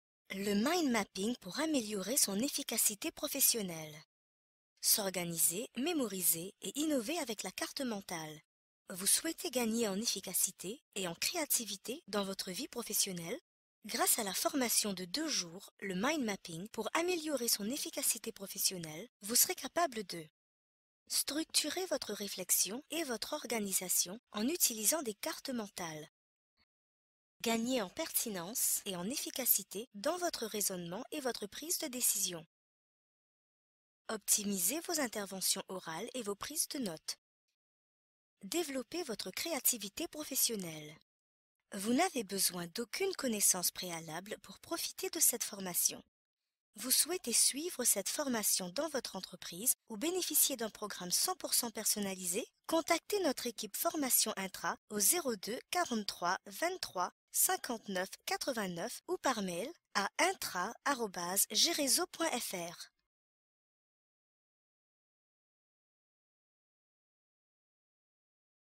Formation "Le Mind Mapping® pour améliorer son efficacité professionnelle"
Vous souhaitez gagner en efficacité et en créativité dans votre vie professionnelle ? Découvrez la formation GERESO "Le Mind Mapping® pour améliorer son efficacité professionnelle"

Objectifs de la formation :
- Structurer votre réflexion et votre organisation en utilisant des cartes mentales.
- Gagner en pertinence et en efficacité dans votre raisonnement et votre prise de décision.
- Optimiser vos interventions orales et vos prises de notes.
- Développer votre créativité professionnelle.

Découvrez le programme complet de la formation "Le Mind Mapping® pour améliorer son efficacité professionnelle"